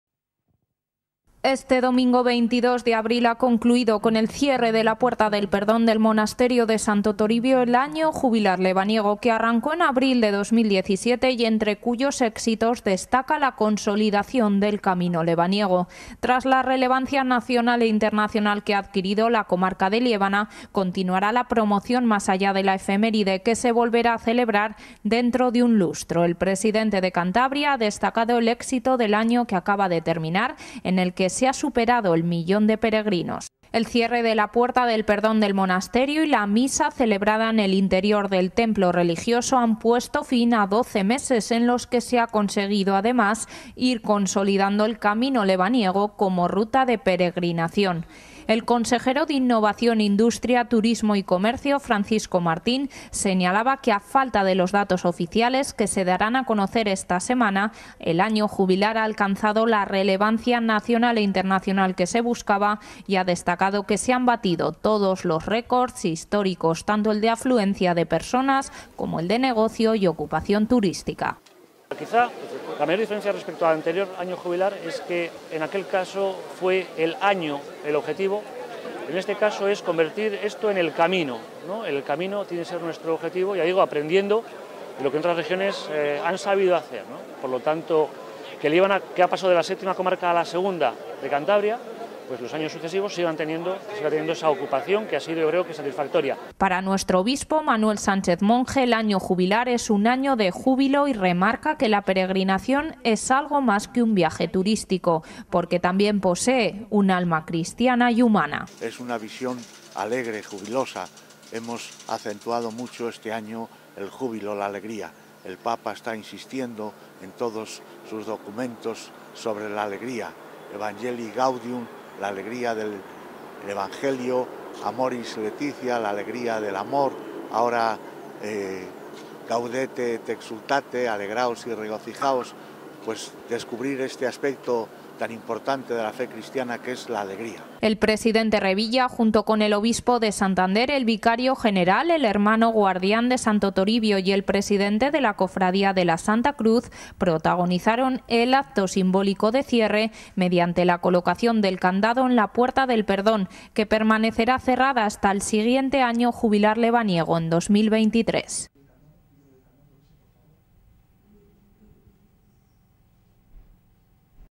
Cierre de la Puerta del Perdón
El domingo, 22 de abril de 2018, el obispo de Santander ha procedido al cierre de la Puerta del Perdón, en un acto acompañado por el cardenal cántabro Carlos Osoro, el obispo de Palencia y los obispos de la provincia